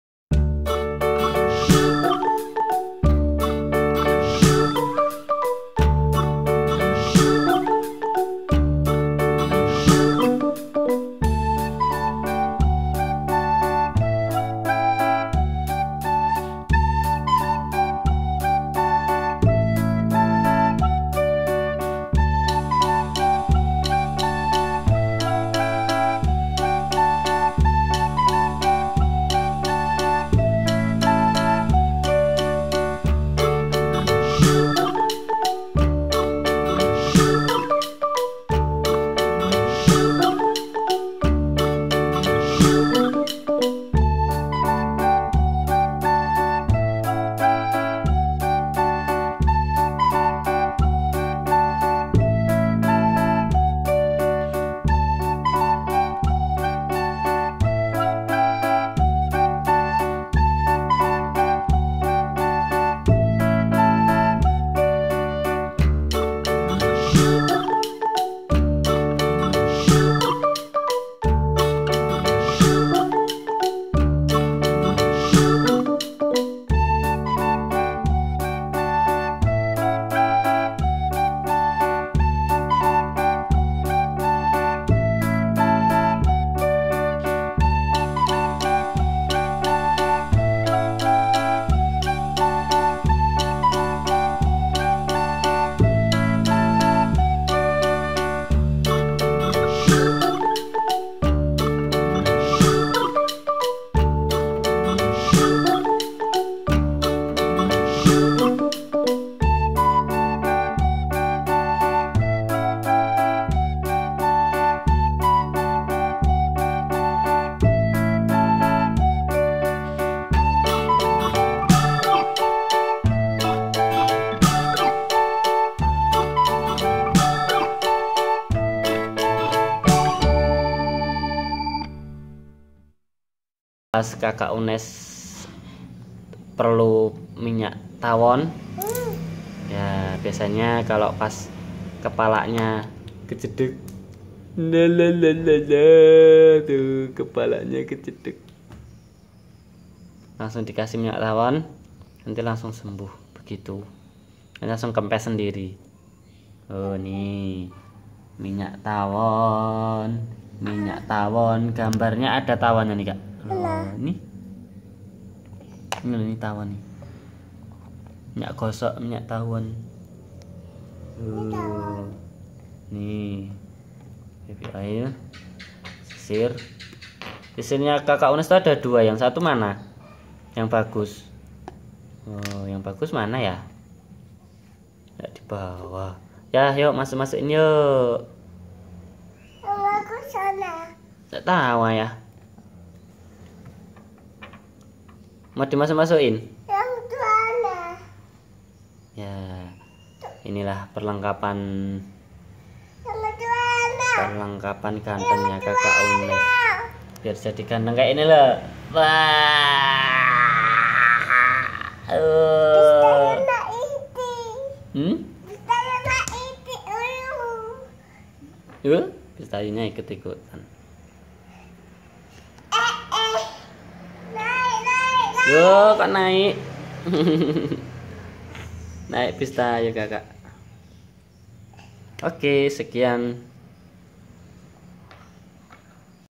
Perlengkapan bayi biar Ganteng KakaUnais
Berikut perlengkapan ganteng kakaunais
- bedak pigeon
- minyak tawon
- minyak telon air mancur
- baby oil cussons